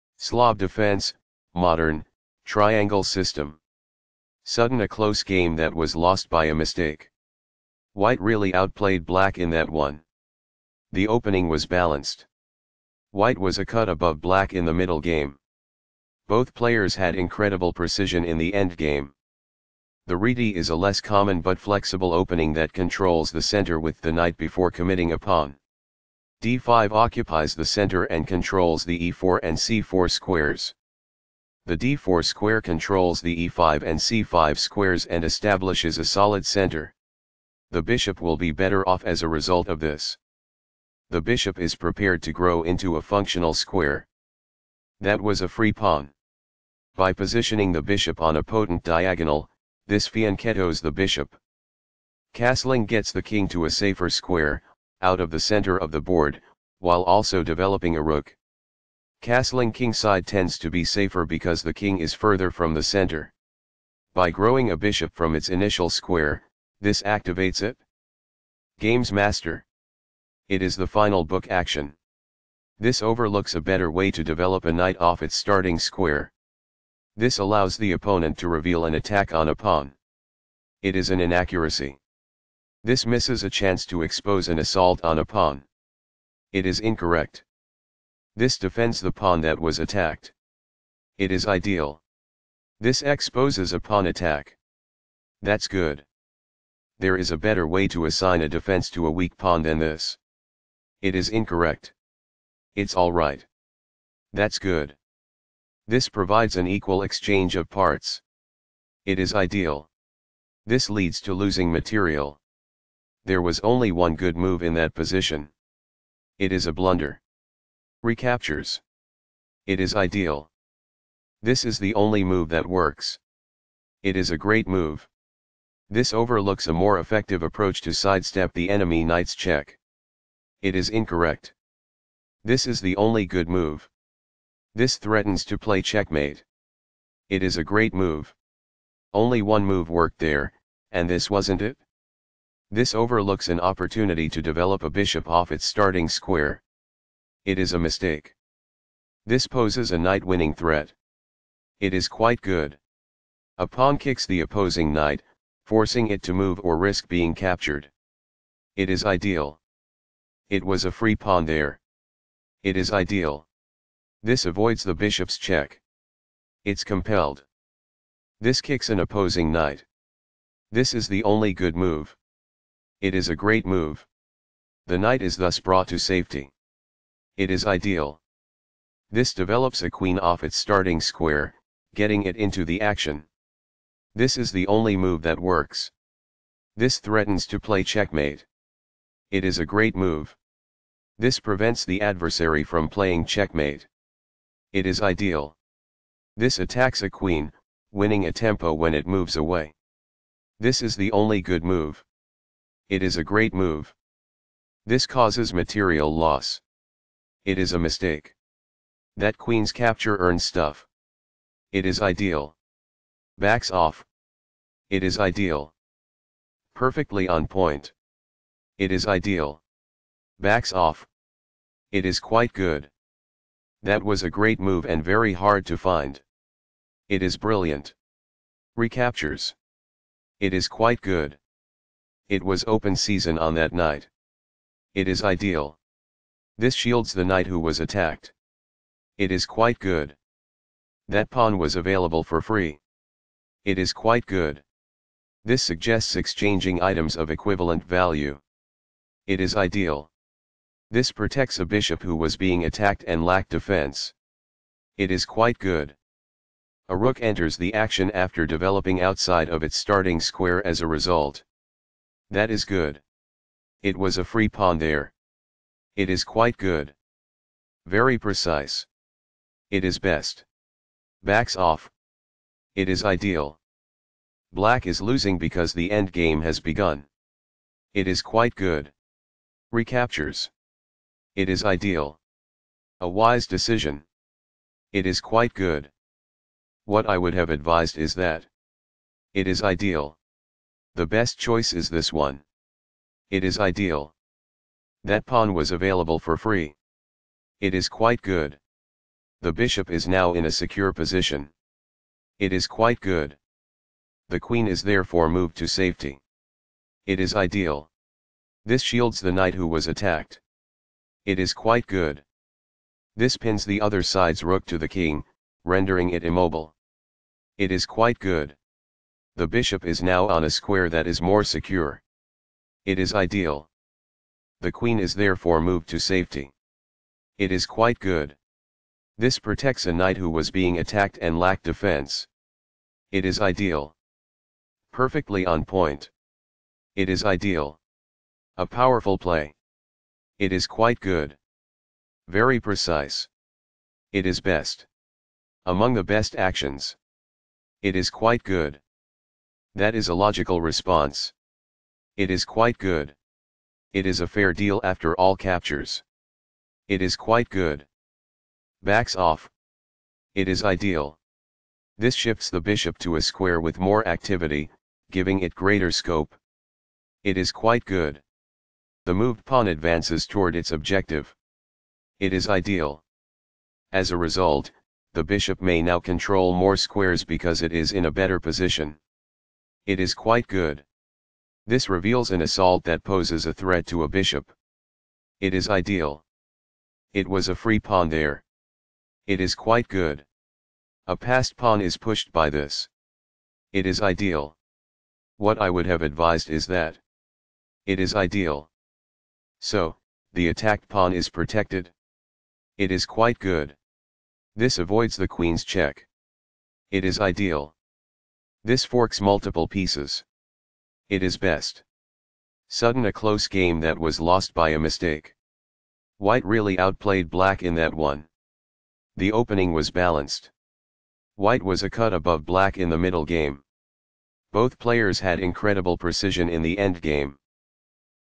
White Grischuk, A., Black Timofeev, Arty,Slav Defense: Modern, Triangle System, Event TCh-RUS, Site
Slav Defense: Modern, Triangle System, Event TCh-RUS, Site Dagomys RUS, Date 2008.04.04, Round 3, White Grischuk, A., Black Timofeev, Arty, Result 1-0, ECO D30, WhiteElo 2716, BlackElo 2664, PlyCount 81, EventDate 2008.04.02